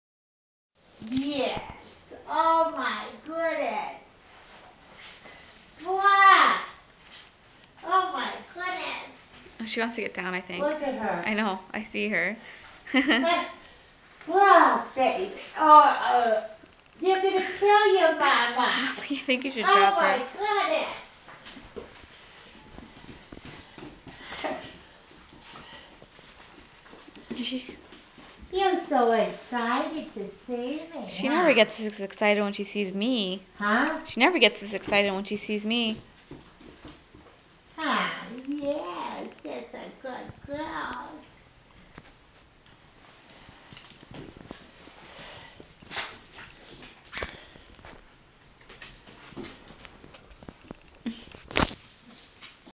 Basenji Loves Her Grandma!
My basenji Kaeda absolutely LOVES her human grandma (my mother) much more than she loves me! She gets super excited whenever she sees her. :)

Also, I'm pretty sure she choked my mom at one point. :x